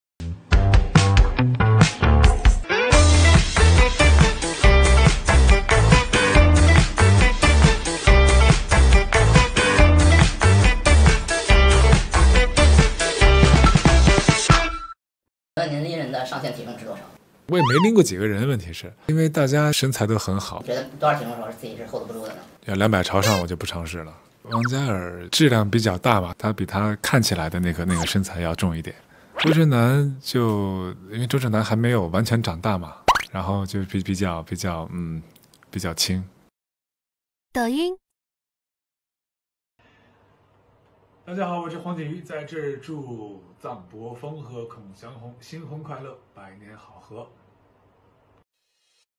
Johnny Huang Jing Yu - short videos of the day (2020.09.07)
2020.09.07 - Short videos featuring Johnny Huang Jing Yu.

1 - ECCO promo
2 - DoRiMe
3 - Congratulations wishes for wedding.